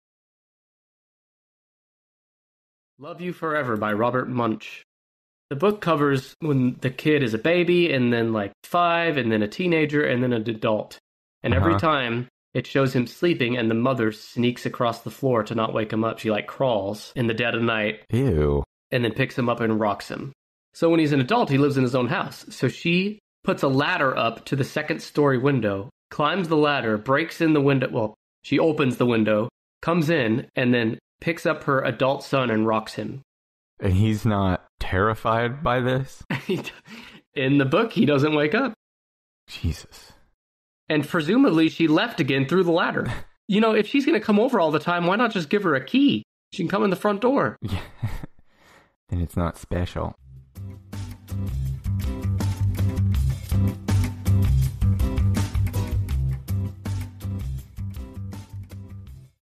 Episode 158 Teaser: Best-Selling Children's Books of All Time
The Grinch. The Giving Tree. Chicka Chicka Boom Boom. Which classic picture book from your childhood has sold more copies than any other? On this episode, Nick has brought the Top 10 Best-Selling Children's Books Ever, and Brandon tries to awaken his inner child to guess all ten.

PLUS: the story behind each story, hot takes and fun facts about each book and each author, helicopter parenting, the movie adaptations, the children's book about the home invader, and much more.